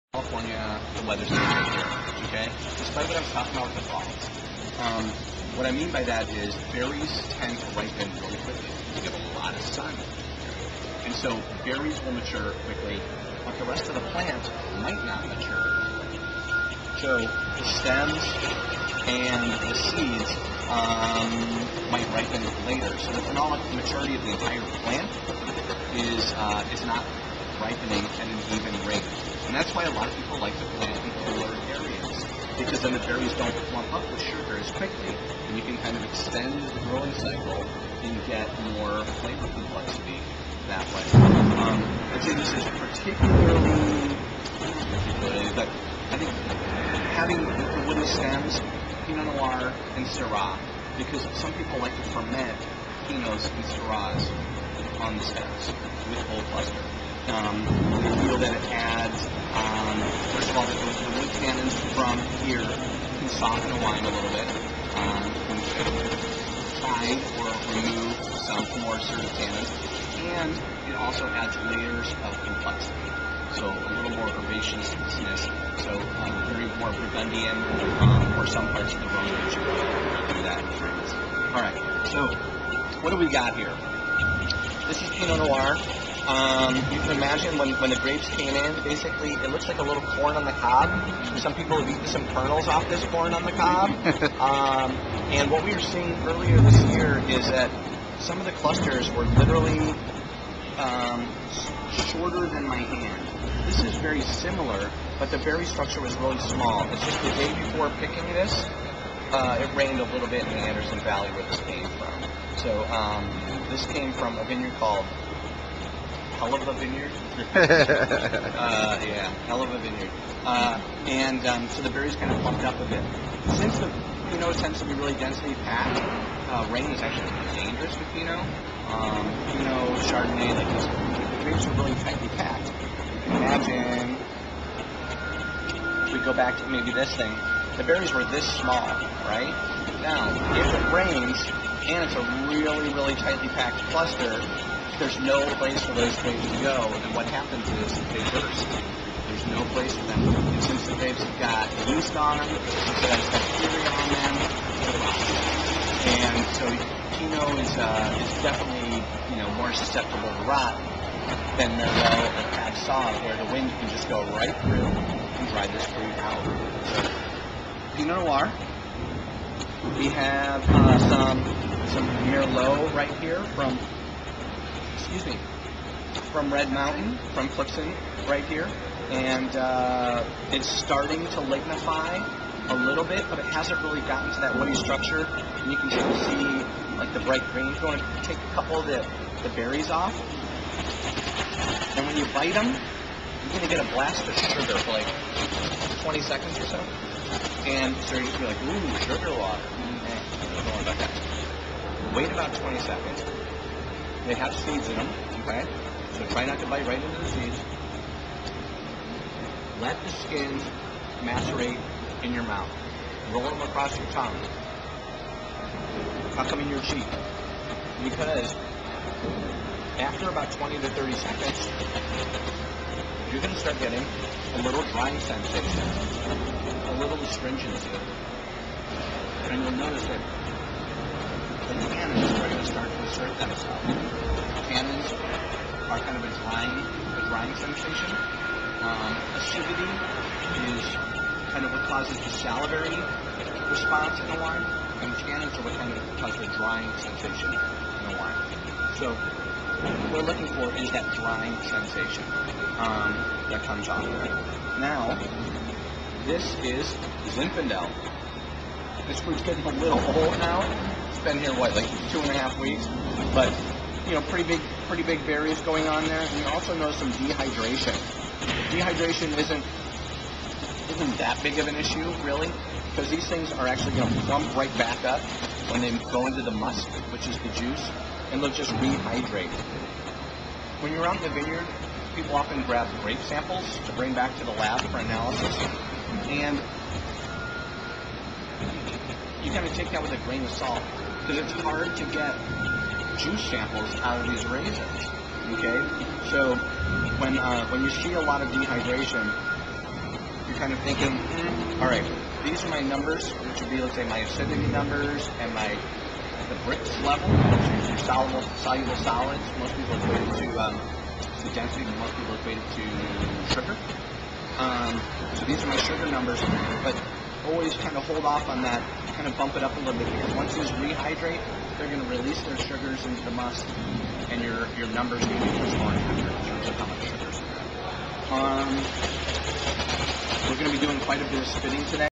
Ripeness of Grapes, Seeds and Stems
Stuart Ake of Crushpad discussing with the Just Grapes Barrel Project 2008 Crew the the importance of ripeness of seeds and stems in addition to the grapes. Sees and stems provide tannin.  Stuart demonstrates the difference in look and size of grapes like Pinot Noir, Merlot and more.